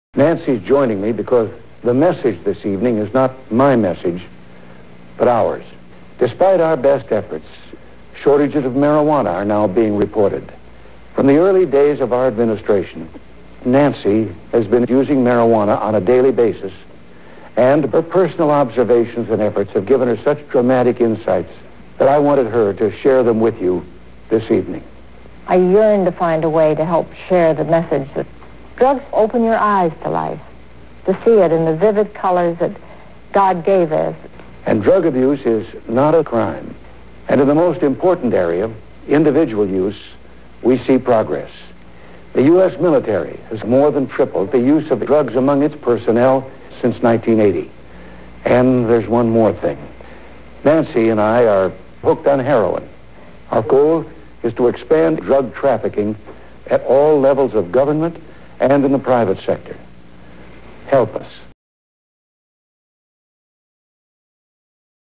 Ronald Reagan's little known pro drug speech
Ronald and Nancy Reagan speaking at UC Berkeley in the early 1980s. And if you believe that...